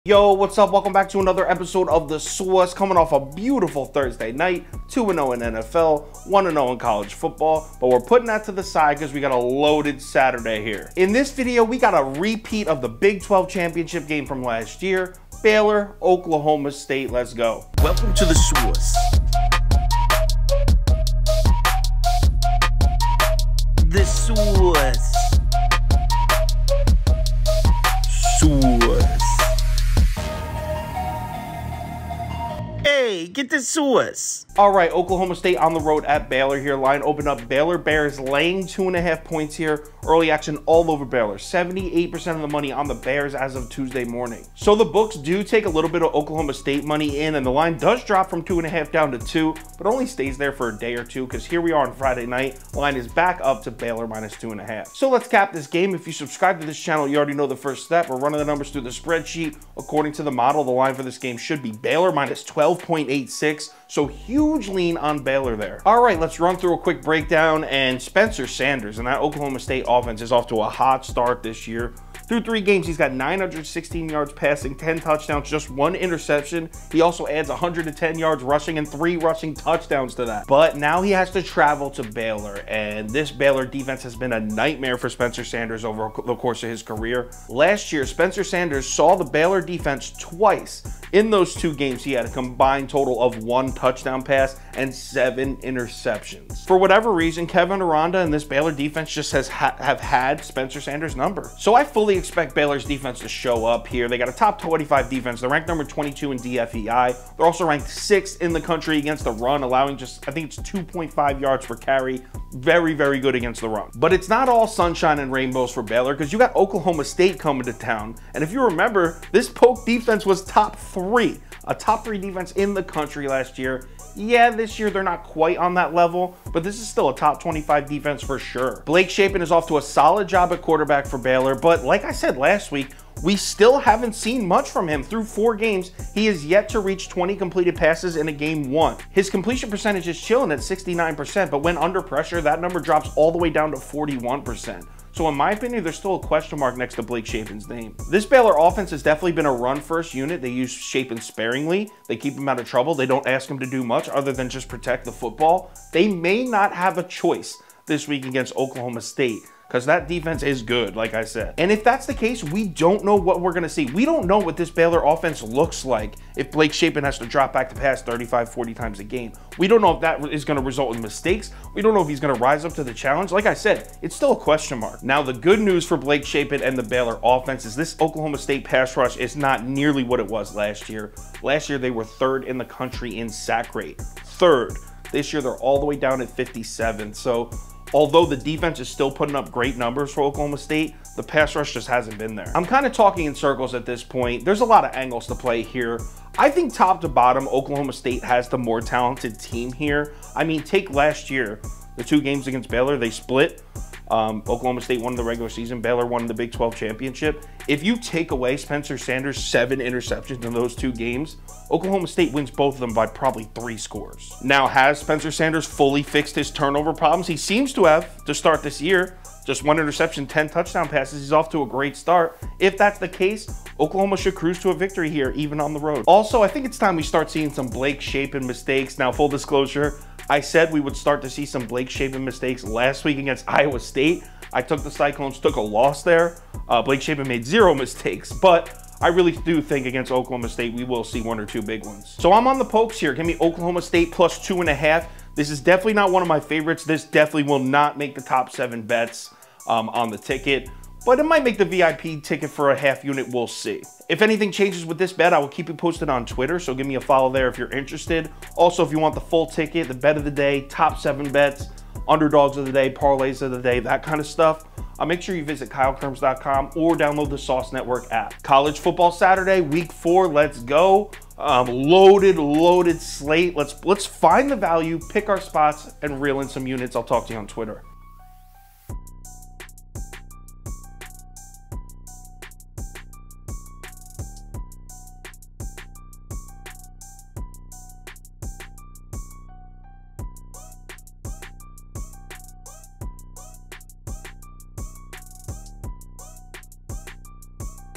Oklahoma State vs Baylor Bears | College Football Week 5 Predictions | The Sauce Network
Oklahoma State vs Baylor Bears | College Football Week 5 Predictions | The Sauce Network | Kyle Kirms

Kyle Kirms, aka "The Sauce", breaks down the Oklahoma State Cowboys @ Baylor Bears game and shares his free pick.

BIG12 SHOWDOWN

@kylekirms - Twitter & Instagram